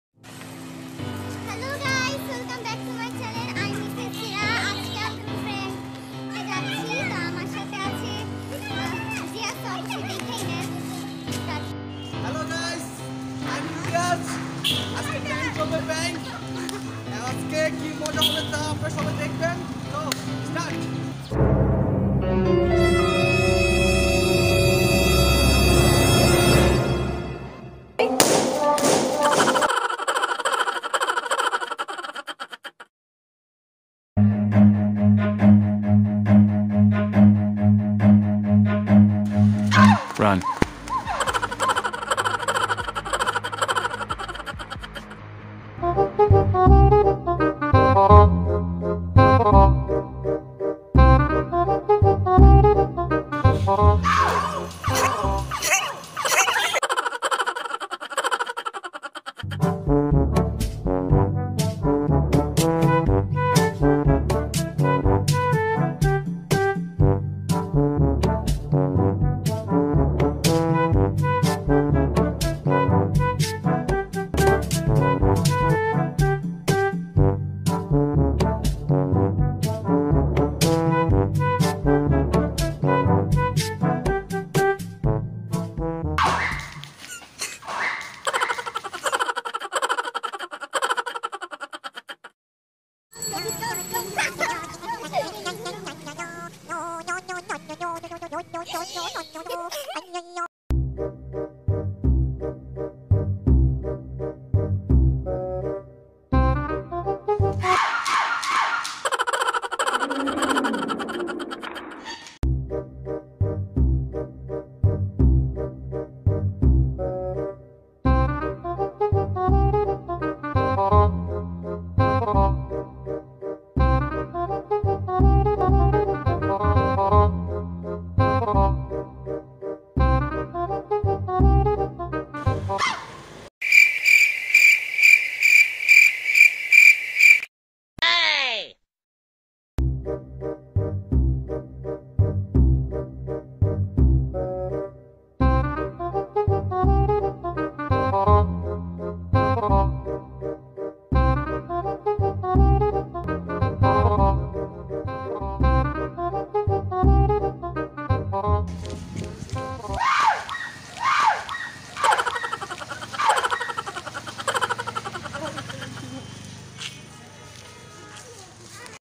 Scream Prank With Public|Funny Prank Short Video By Mrs Hira's Channel|Prank Videos in BD|Fun Video
Welcome to Mrs Hira's Channel ❤️🥳

Scream Prank With Public By Mrs Hira and Imtiaz Bin Munna